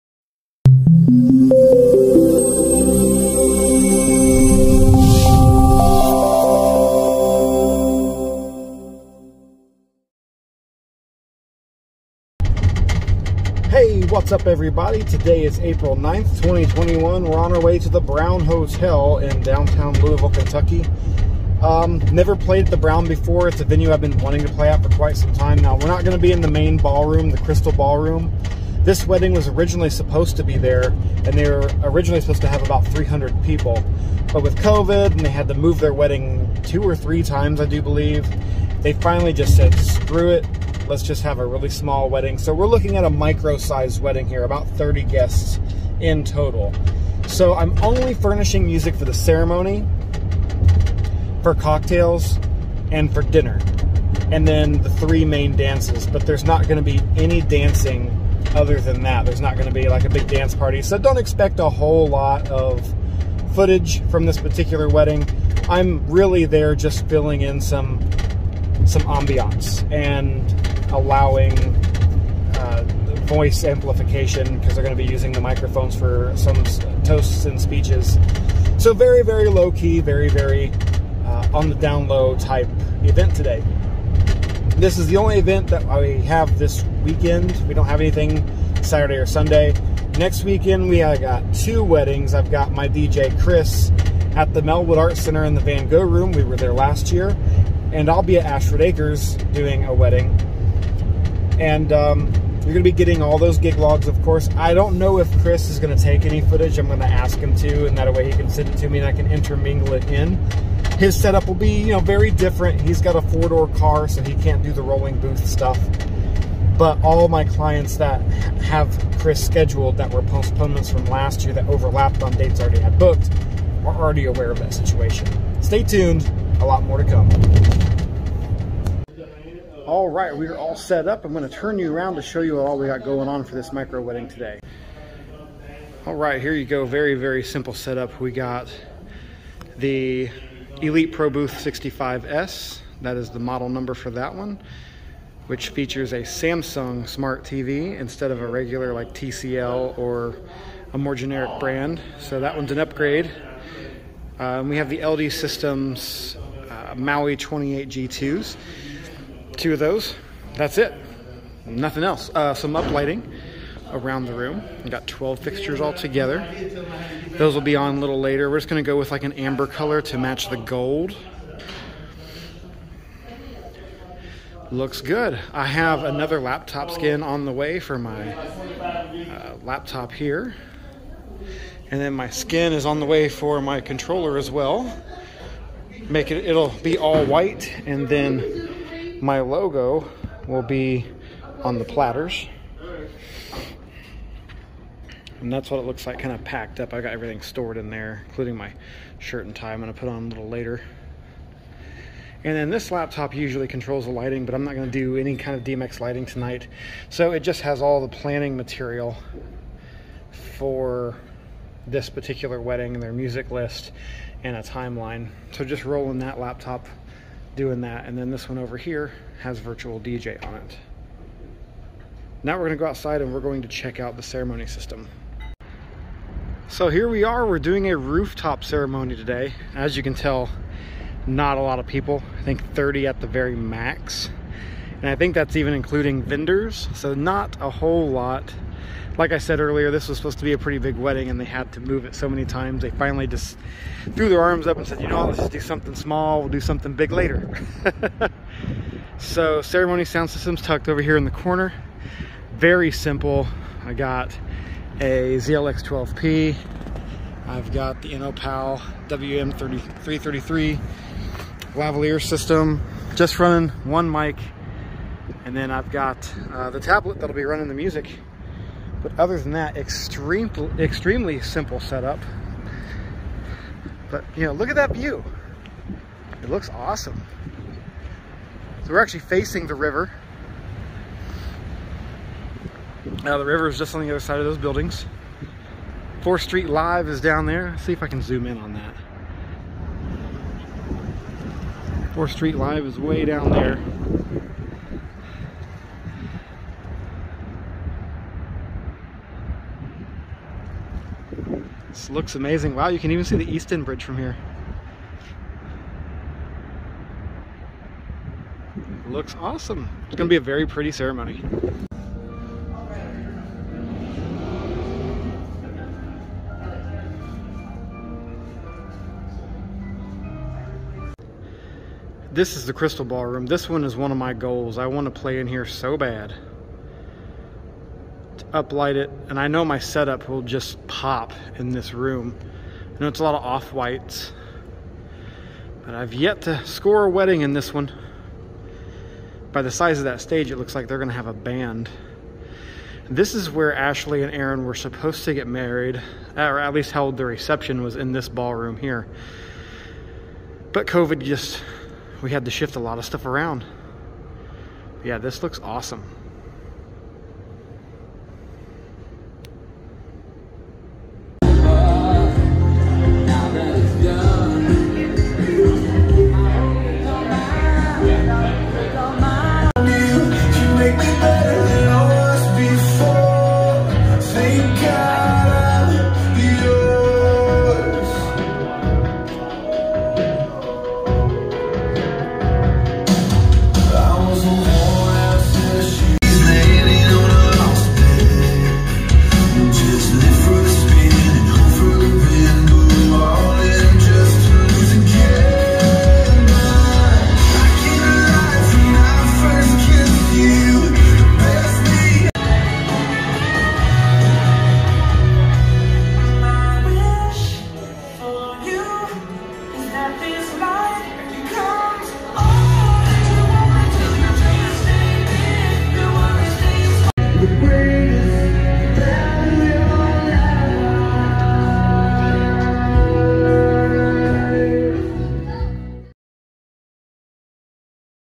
Wedding Gig Log | Micro Wedding | The Brown Hotel | Louisville, Kentucky
A very small. Wedding with no dancing other than formals at the Brown Hotel downtown Louisville.